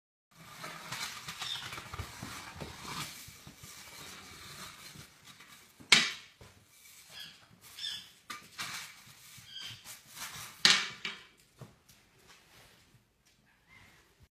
Adorable Raccoon Helper
Have you ever seen a more adorable raccoon helping out with household chores?! :)